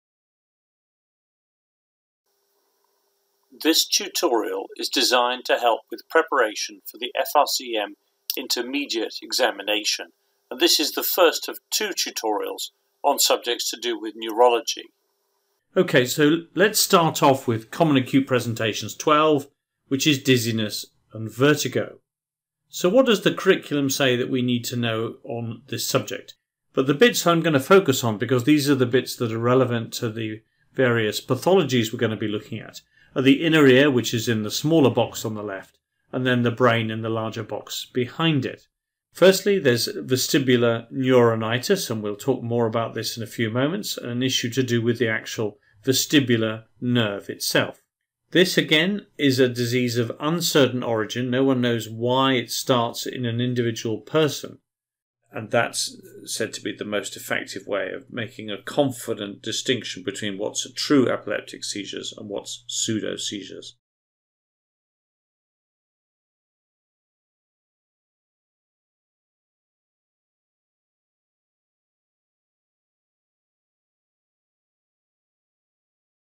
MRCEM Intermediate SBA Neurology 1 Preview
Here is a preview of our new Neurology 1 webinar, which is included in our MRCEM Intermediate SBA online revision resources.